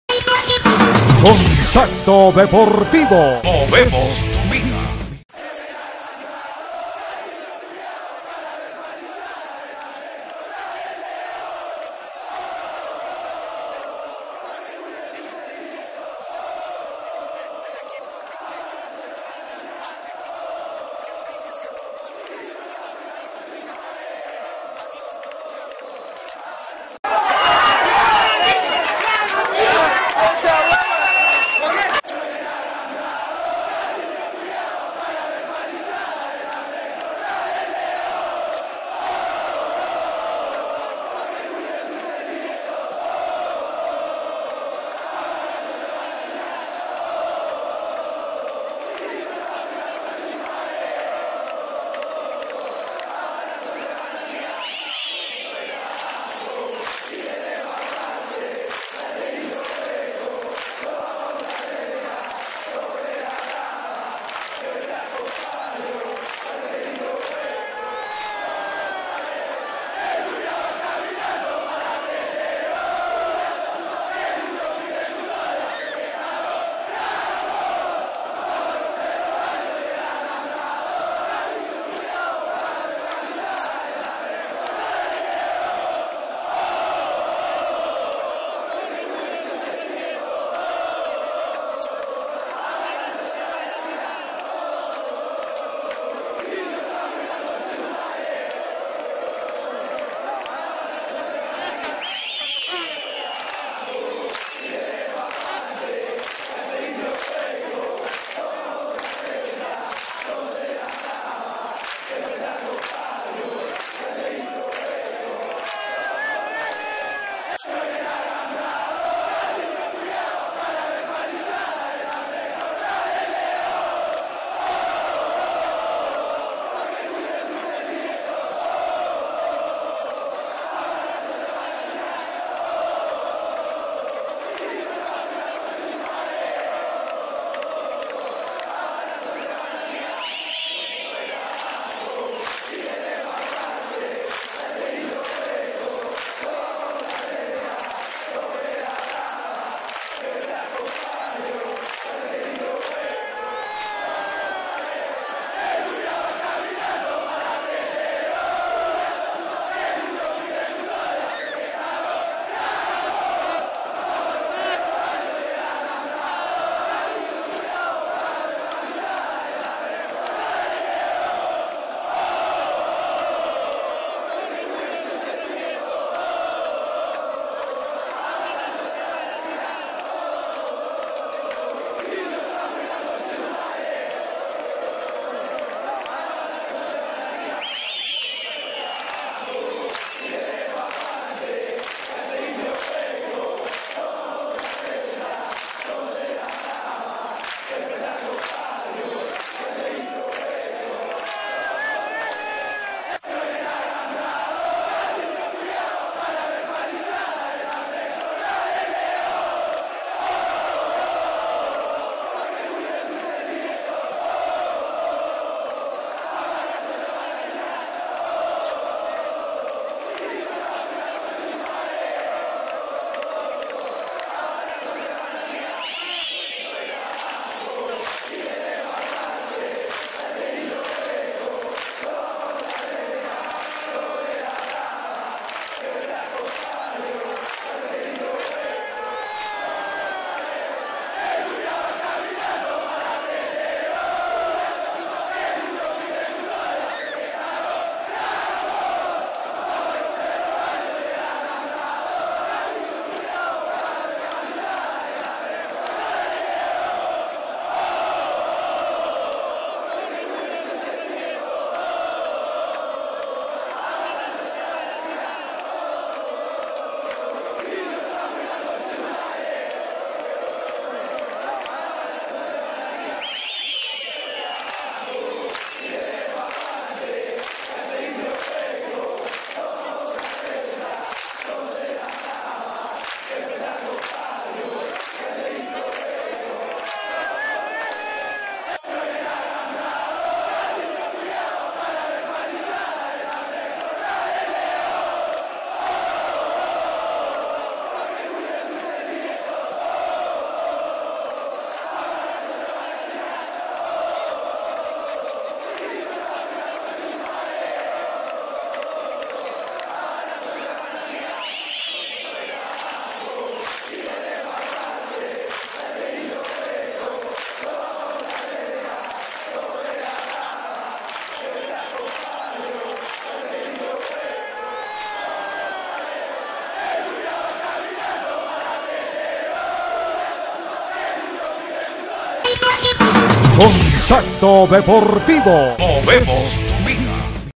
PENALES JORGE ENRIQUEZ VS UNIVERSITARIO
El equipo de Jorge Enriquez le dejo en el camino al Universitaro de Píntag en los tiros penales y va al 5to. Tirulo del Campeonato de Campeones de Ligas Rurales